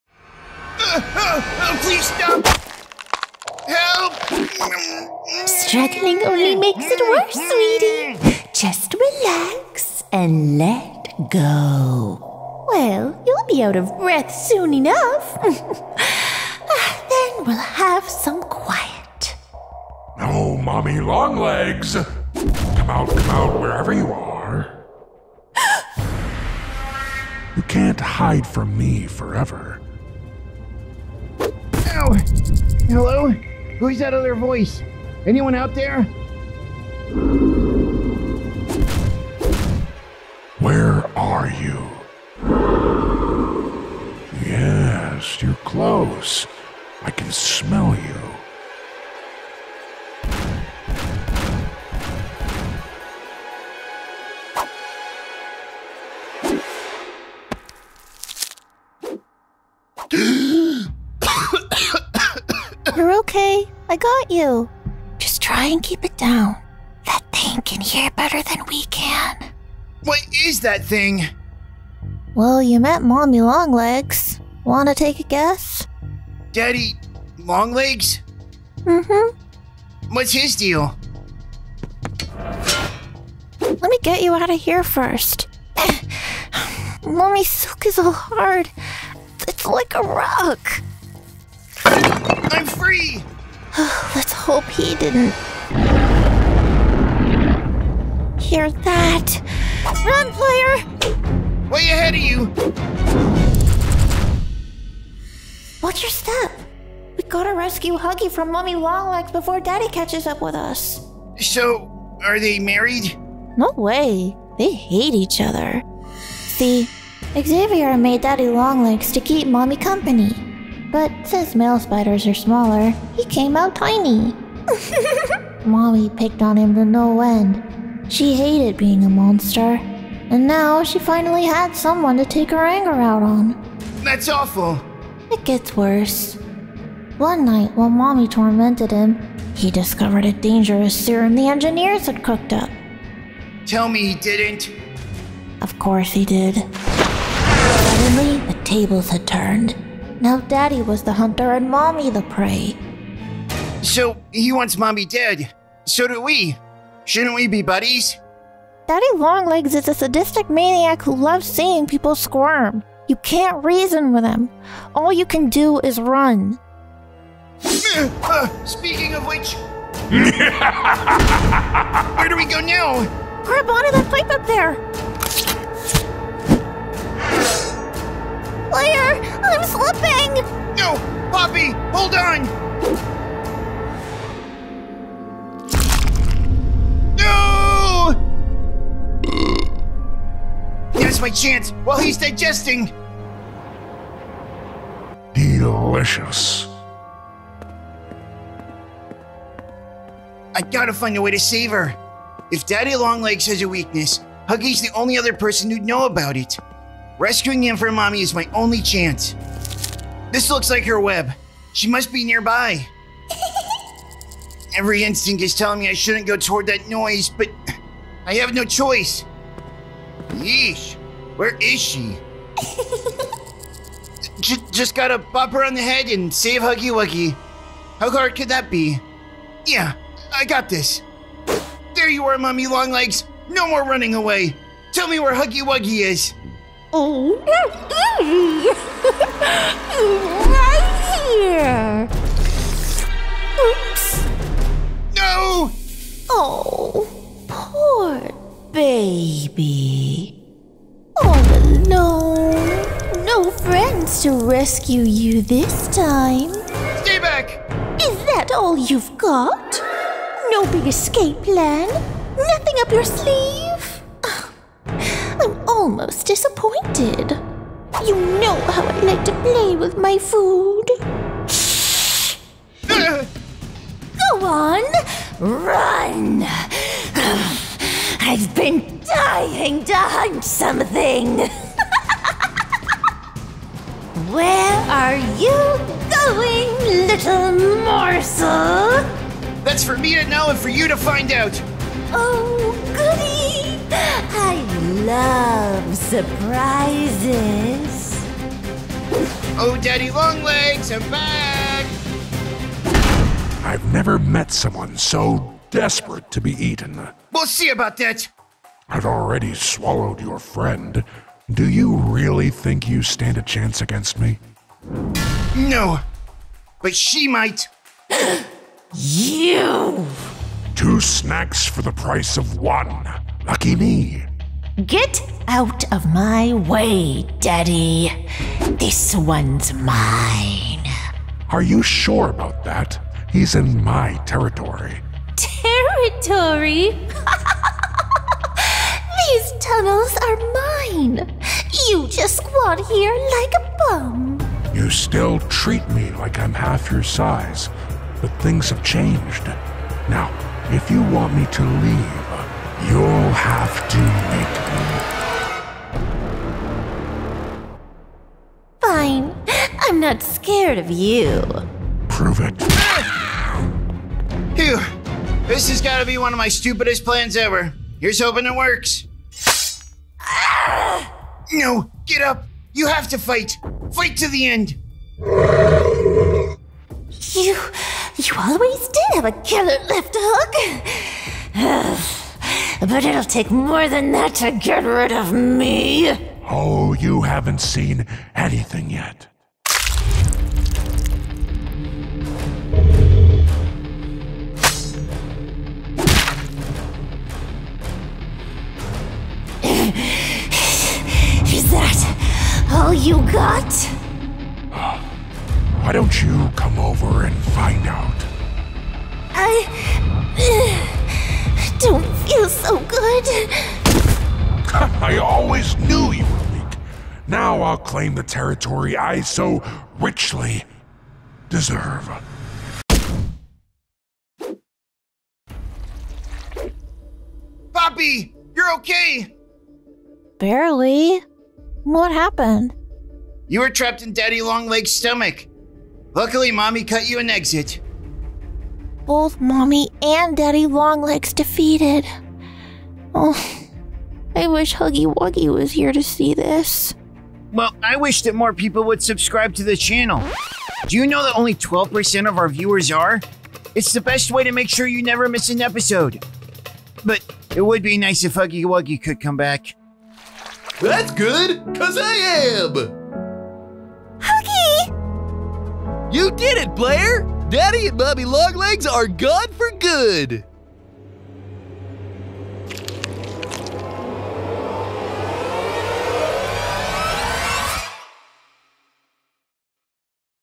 LEGEND of DADDY LONG LEGS... (Cartoon Animation)
Mommy Long Legs wraps Player in a deadly spider web cacoon, but before she can drag him back to her awful lair, Daddy Long Legs appears! Daddy Long Legs is a giant red and black spider created by Playtime Co to be Mommy Long Legs' companion. Now in order to free Poppy and Huggy Wuggy, Player must get them to fight! Who will win? Watch the whole episode to find out!